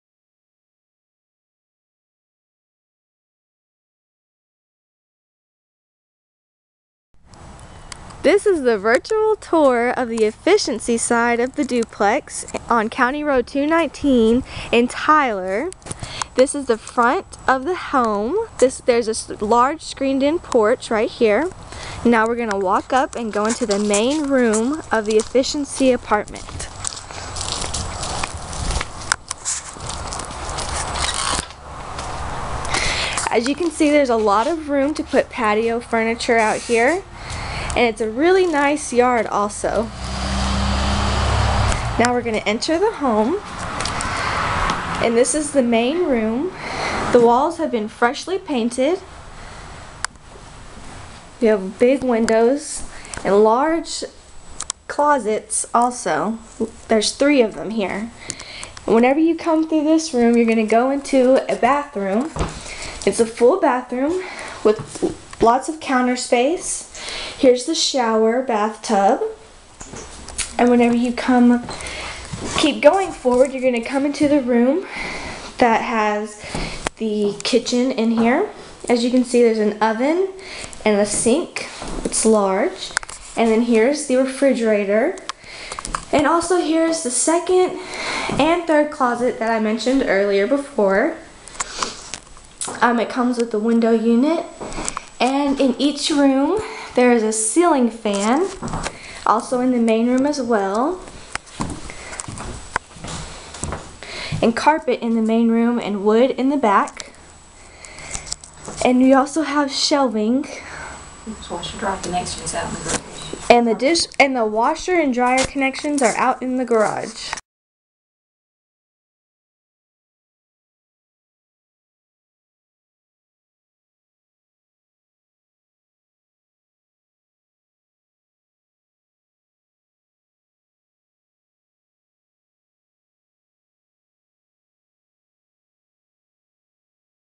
Efficiency Apt Tyler, TX
This is a virtual tour of the efficiency side of the duplex in Tyler, TX. Thank you!